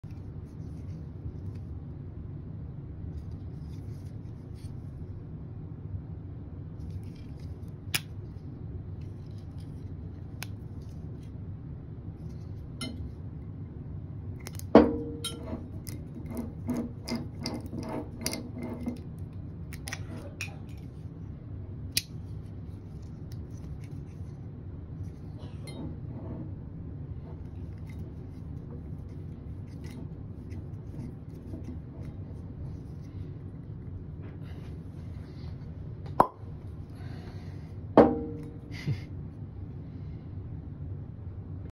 Ruike Corkscrew Works!
It's a chunker, but I like the Ruike M42-G.

Host: Bob "The Knife Junkie" DeMarco